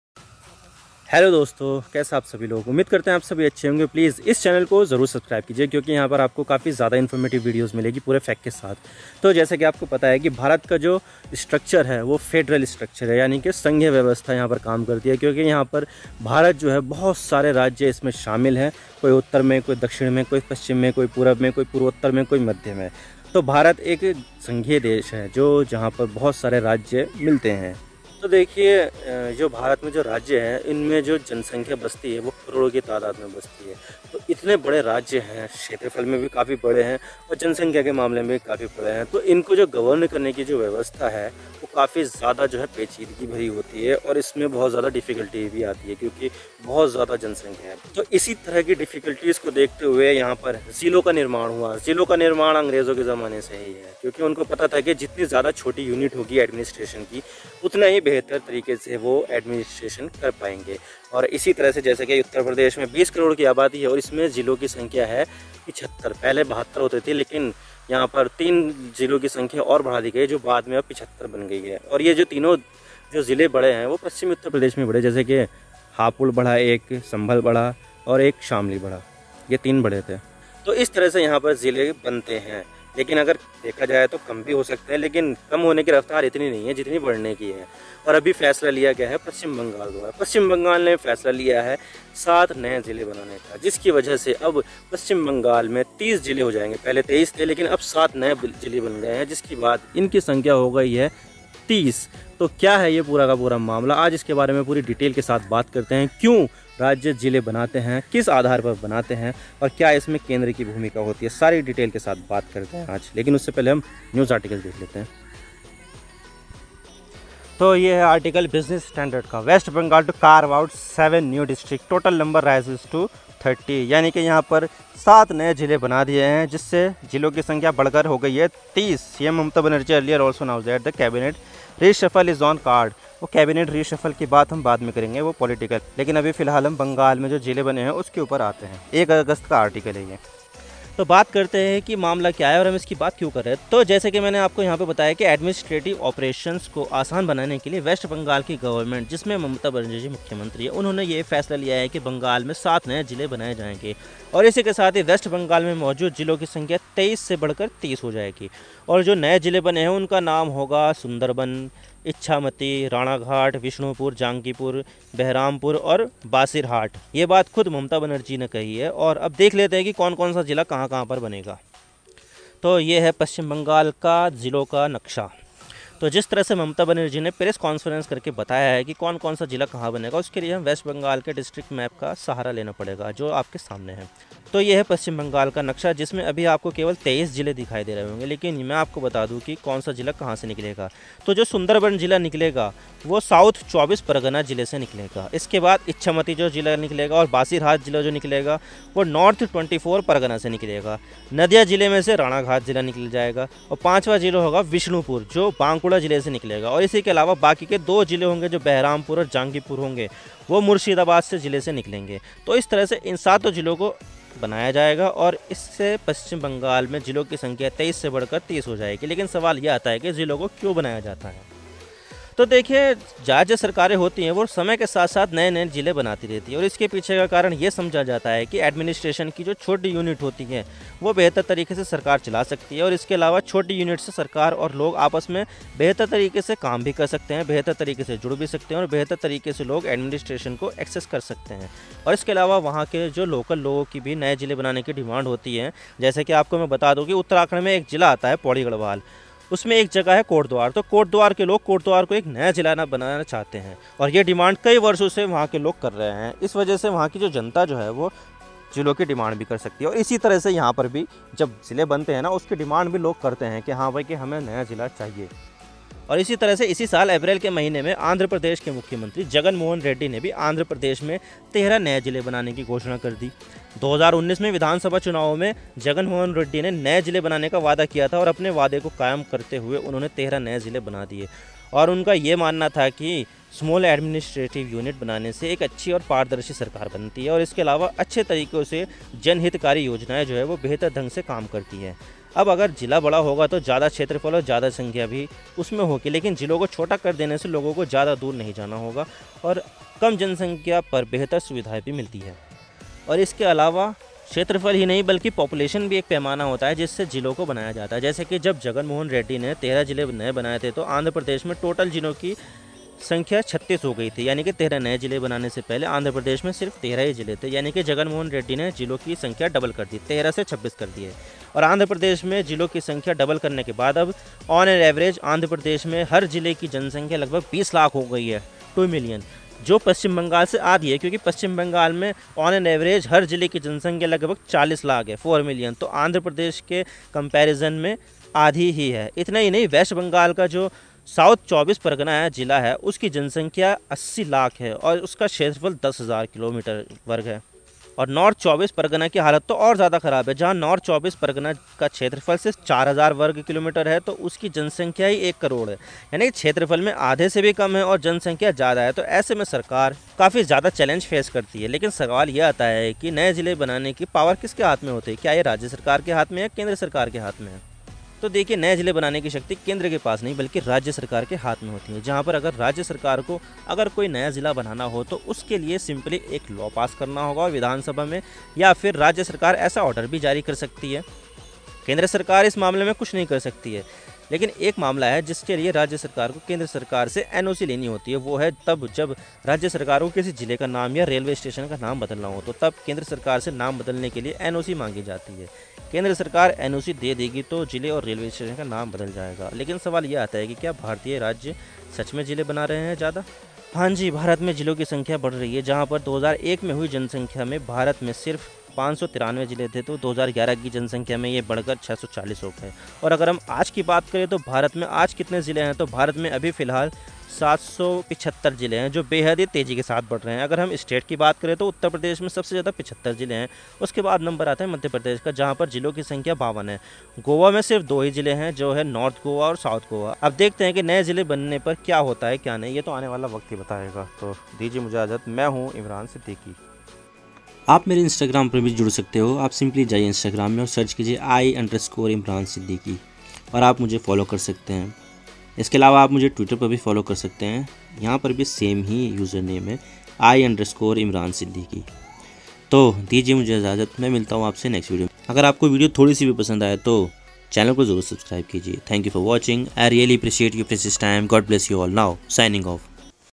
Explained: 7 New District in West Bengal | Why Number of District Keep Rising in India? | InfoMagnet
Hi Friends, In this video We will talk about why Mamta Banerjee add up 7 new district in west Bengal? why a small administration unit works better? why number of district keep rising in india? let's see

पश्चिम बंगाल में ममता बनर्जी ने बनाये 7 नए ज़िले जिससे ज़िलों की संख्या 23 से बढ़कर हुई 30। क्यों एक छोटी प्रशासनिक इकाई बेहतर प्रशासन के लिए अच्छी मानी जाती है? कैसे सरकार ज़िलों का निर्माण करती है? क्या भारत मे ज़िलों की संख्या निरंतर बढ़ती जा रही है? जानिए सबकुछ इस वीडियो में

Host: Imran Siddiqui
Voice: Imran Siddiqui
Edited By: Imran Siddiqui

All Advice, Suggestion, Criticism and Appreciation Are Welcome